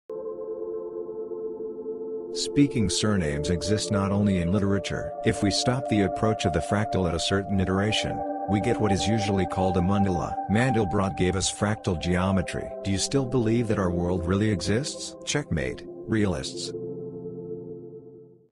Speaking surnames | Mandelbrot Set Fractal Jokes
Speaking surnames exist not only in literature. If we stop the approach of the fractal at a certain iteration, we get what is usually called a mandala. Mandelbrot gave us fractal geometry. Do you still believe that our world really exists? Checkmate, realists!